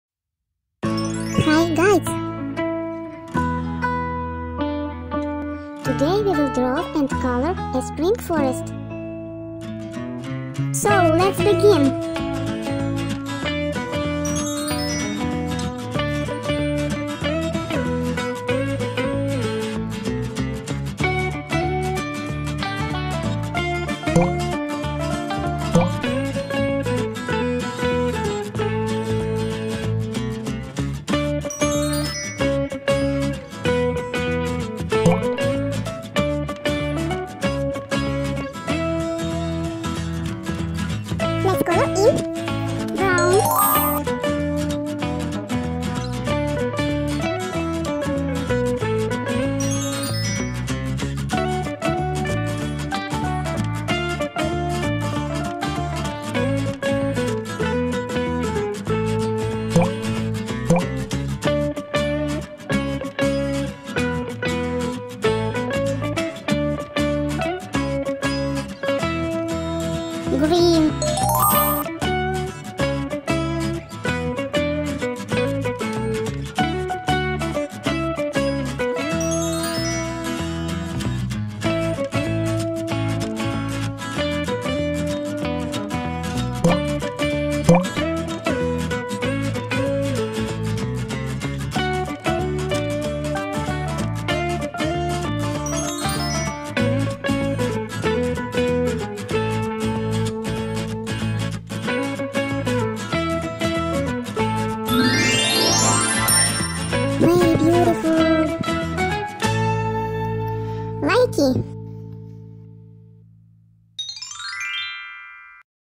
HOW TO DRAW A FOREST FOR KIDS🎨🎨
🎨 How to draw and color cartoon cute forest. Cartoon coloring for kids. Learn how to draw easy drawings for kids. Today we will learn how to draw a forest 🎨